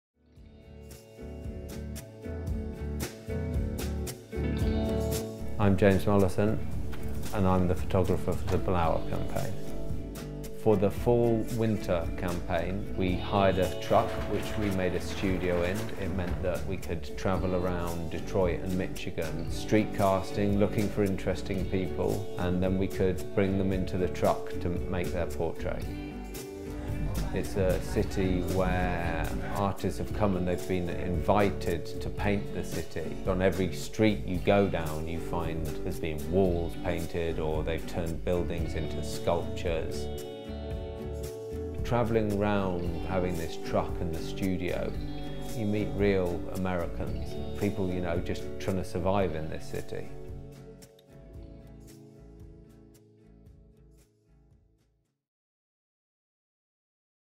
FW 18/19 | BLAUER’S AMERICAN PORTRAITS HEADS TO MICHIGAN
Once again, photographer James Mollison rented a van, turned it into a travelling photography studio and set off on a search for men, women and children who could wear the garments of the new collection and embody the most authentic spirit of America.
This time it heads to Michigan, on the northern edge of the United States.

From the rugged wilderness of the Upper Peninsula to Detroit, the symbol of the creative energy that fuelled the American dream, Michigan is a place of contrast, that still breathes the legends of the Native Americans and the sense of adventure of the European explorers.